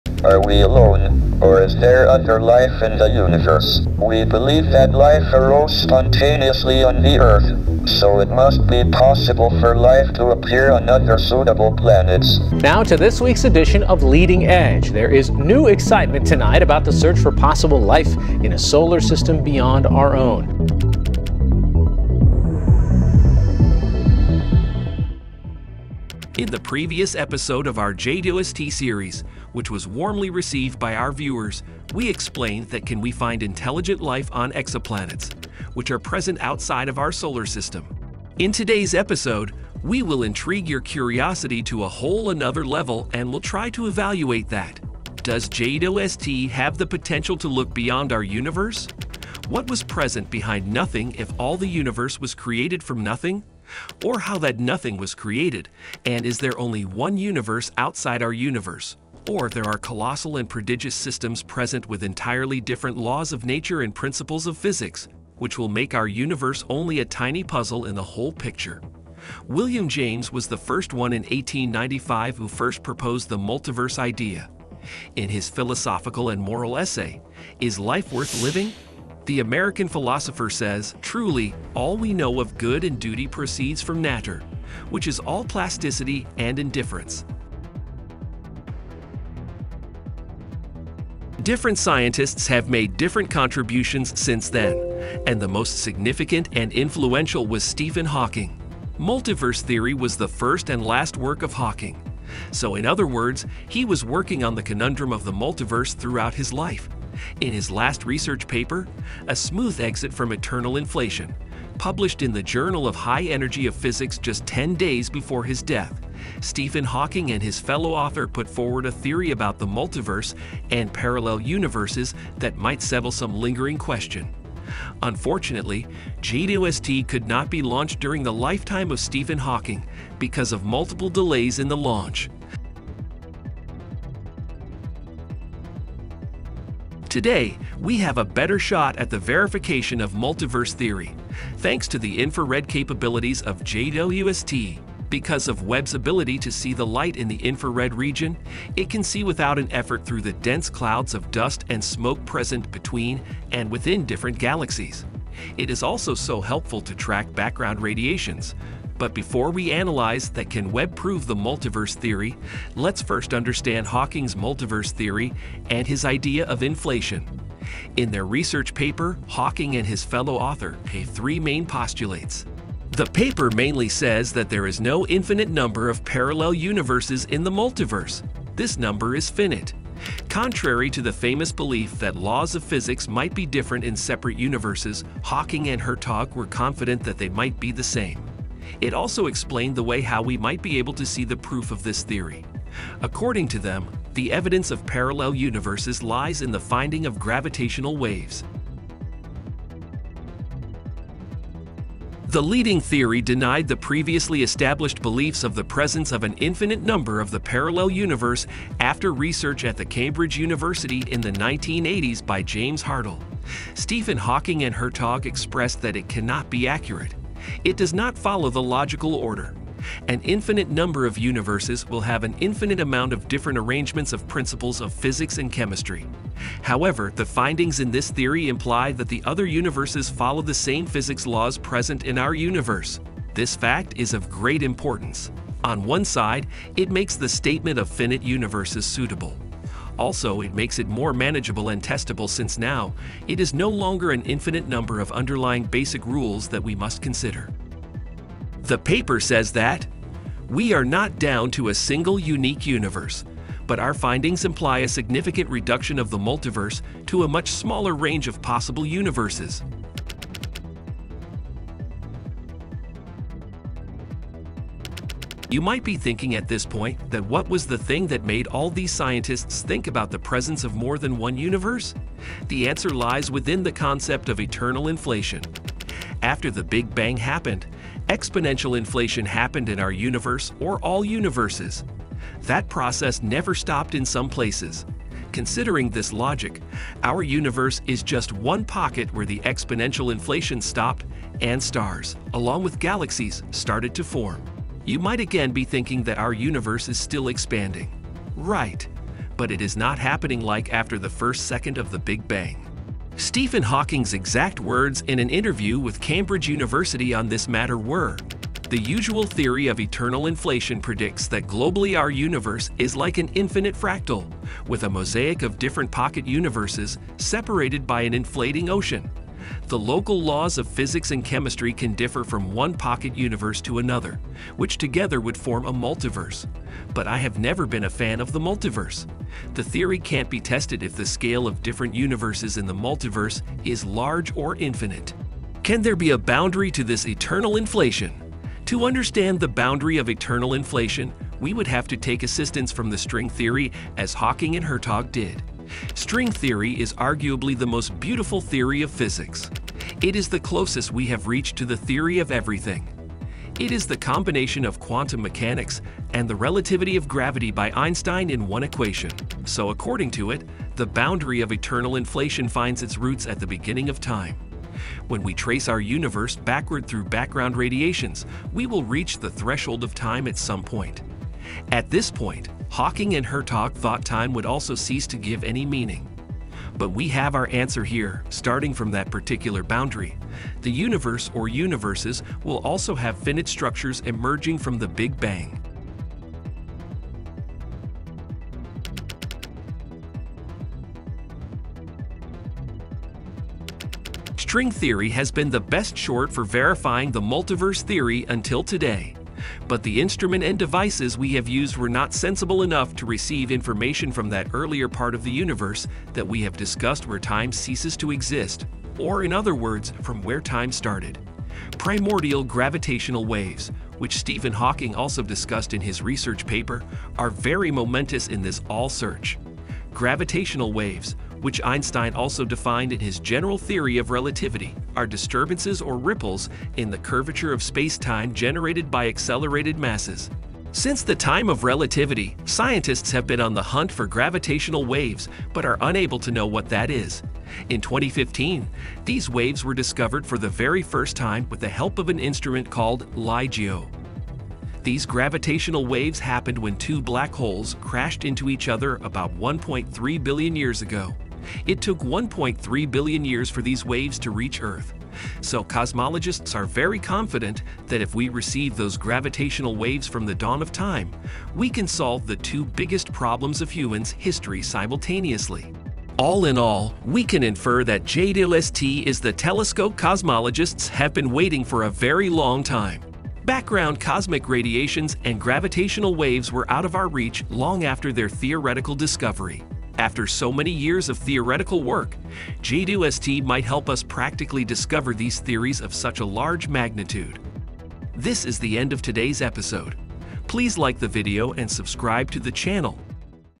Can James Webb Telescope Really Prove HAWKING’s Multiverse Theory?
In the previous episode of our JWST series, which was warmly received by our viewers, we explained that can we find intelligent life on exoplanets, which are present outside of our solar system. In today’s episode, we will intrigue your curiosity to a whole another level and will try to evaluate that. Does JWST have the potential to look beyond our universe? What was present behind nothing if all the universe was created from nothing? Or how that nothing was created and is there only one universe outside our universe, or there are colossal and prodigious systems present with entirely different laws of nature and principles of physics, which will make our universe only a tiny puzzle in the whole picture. William James was the first one in 1895 who first proposed the Multiverse idea. In his philosophical and moral essay, “Is life worth Living?” the American philosopher says,” Truly, all we know of good and duty proceeds from nature… [which] is all plasticity and indifference – a moral multiverse, as one might call it” and this is how a new word was born.